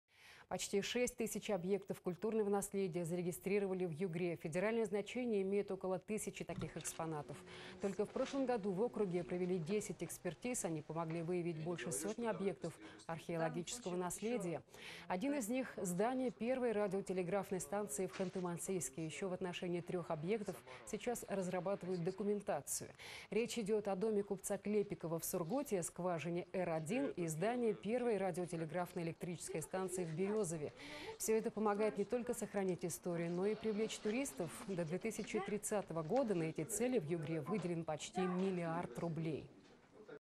Скважину в Берёзовском районе хотят внести в реестр культурного наследия
Почти 6 тысяч объектов культурного наследия зарегистрировали в Югре. Федеральное значение имеют около тысячи таких экспонатов.